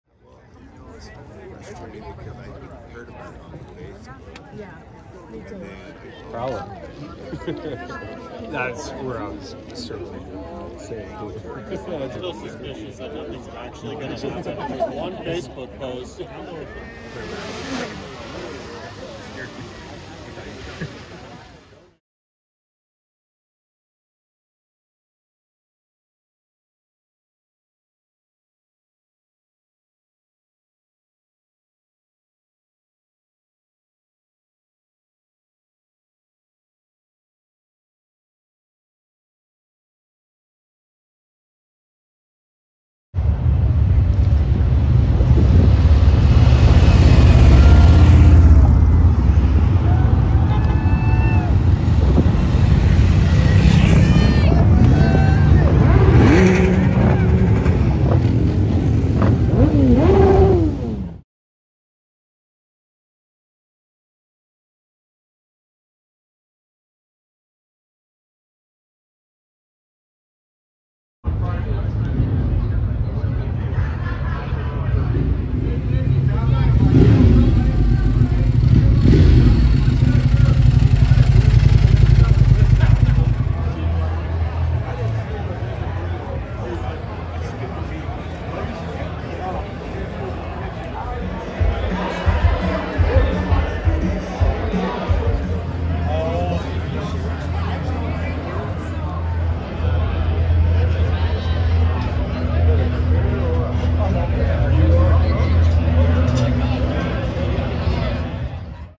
Evil Knievel 50 year anniversary celebration
People gather from all over to celebrate Evel Knievel’s 50th anniversary jump attempt celebration in Twin Falls, Idaho.